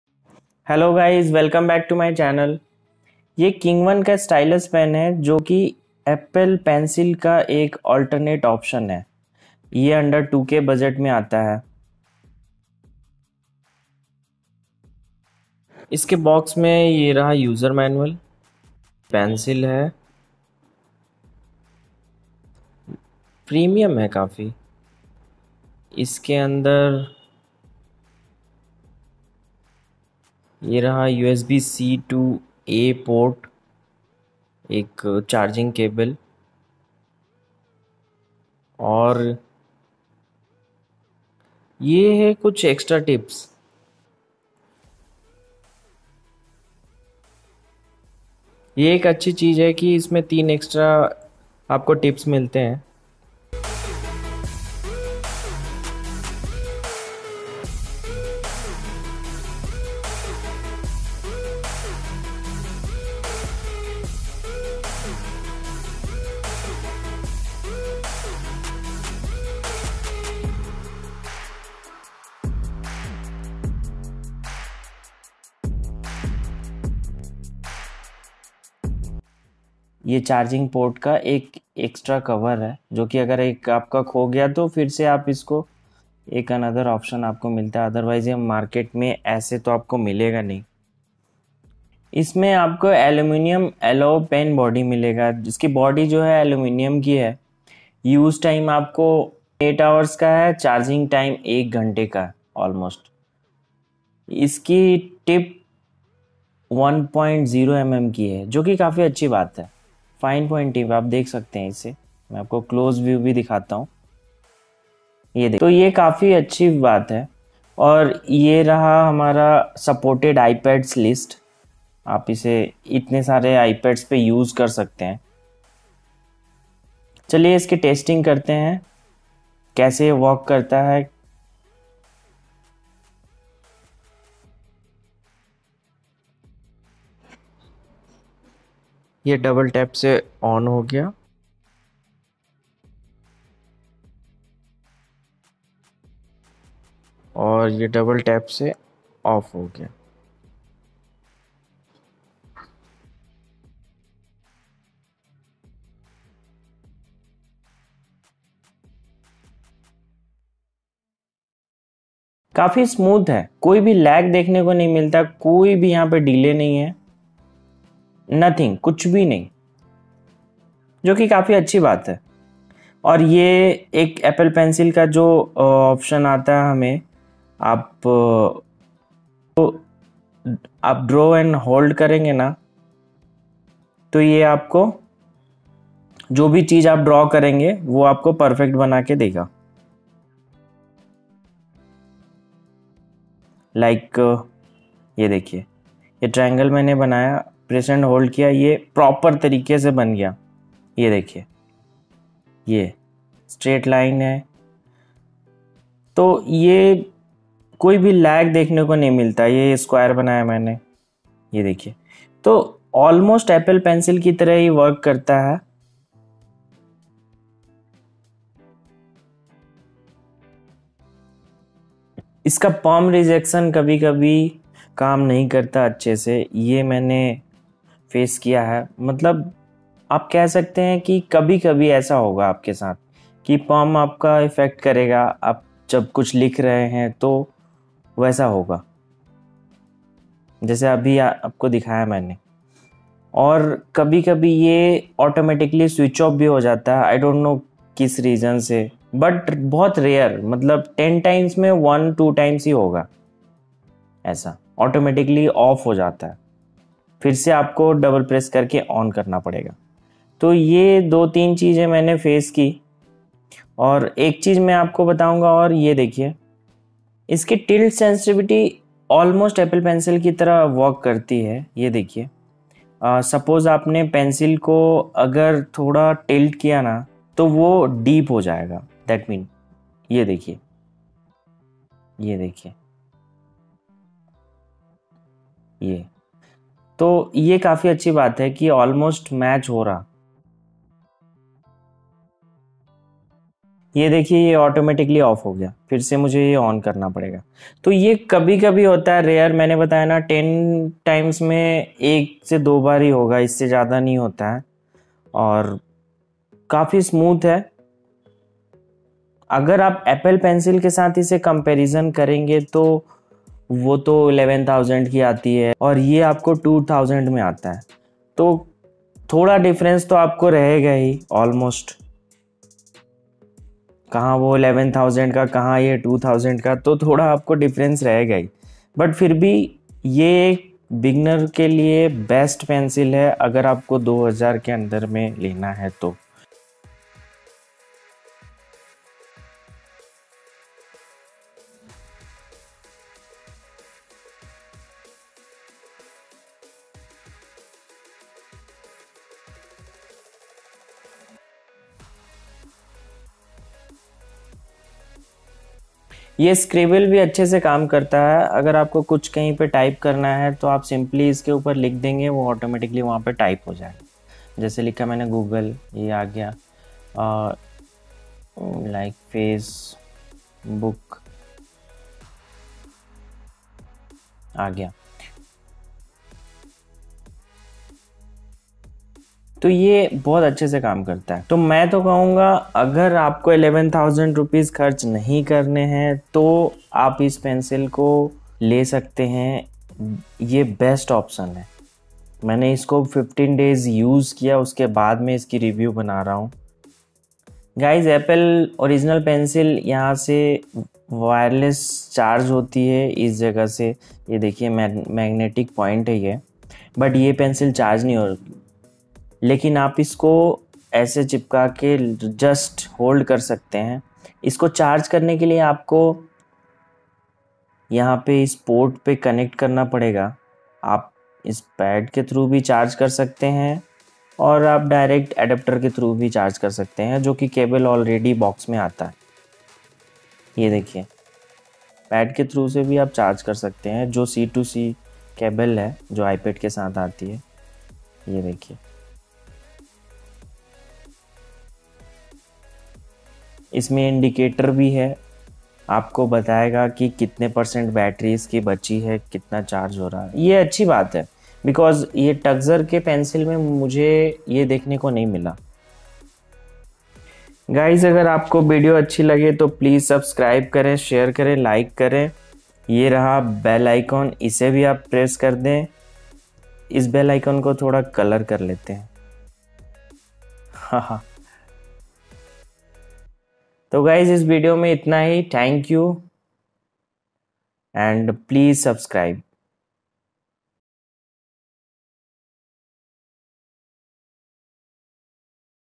Best Apple Pencil Alternative | under 2k | Kingone Stylus Pen For ipads
Apple pencil alternative, Kingone stylus pen for ipads.

Kingone Stylus pen (ipad pencil)
Ipad air 4th gen screen guard
Ipad back cover

OnePlus Buds Z2 Bluetooth Truly Wireless Earbuds with mic Pearl White